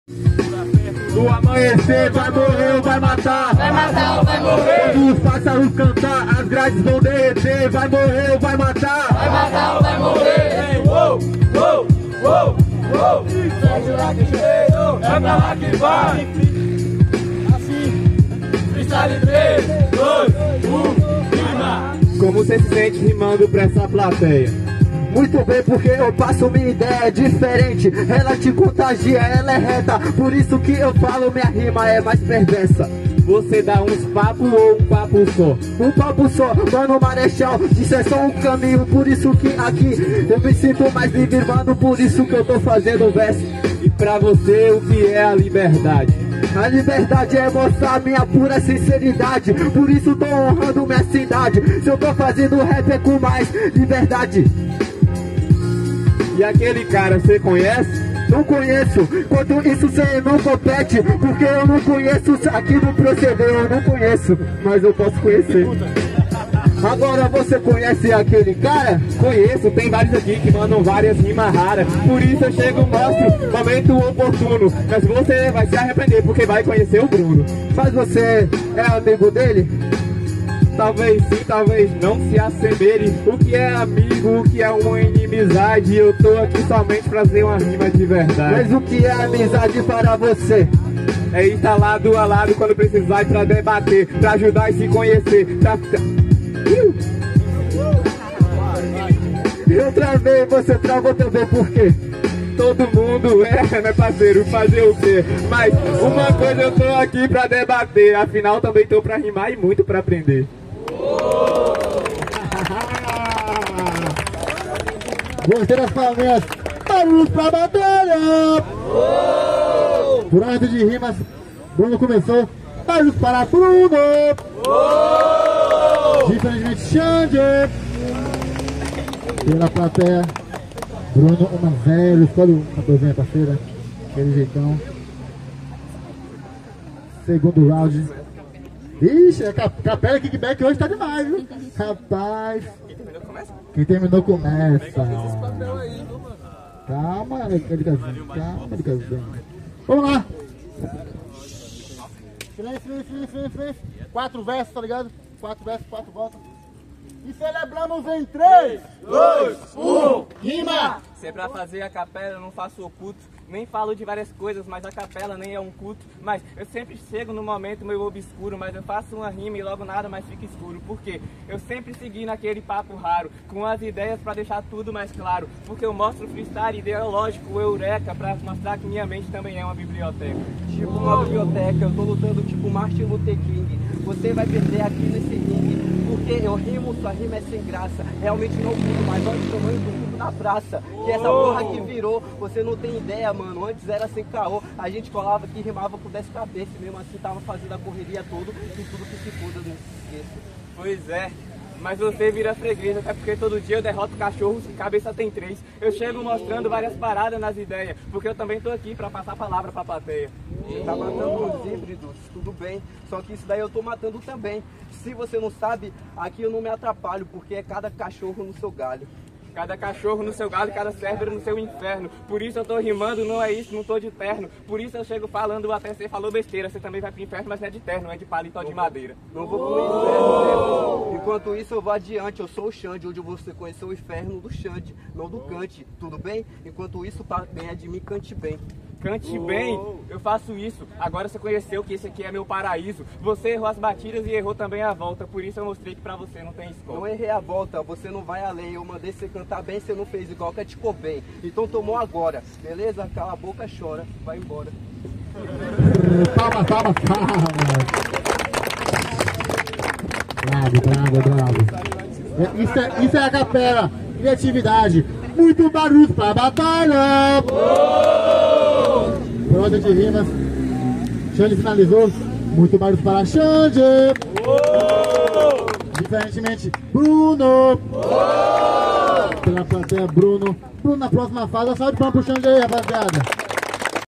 BRUNO X XANDY - PRIMEIRA FASE - BATALHA DO COMPLEXO - 222° EDIÇÃO - BA
A Batalha do complexo é um movimento cultural da cidade de Porto Seguro -Ba. Movimento que vem solidificando cada semana com freestyle estilo livre na praça do bairro Frei Calixto. Batalha que vem ocupando espaços em escolas e universidade da cidade!

BATALHA TODO DOMINGO NA PRAÇA DO TRABALHADOR - BAIANÃO (PORTO SEGURO-BA)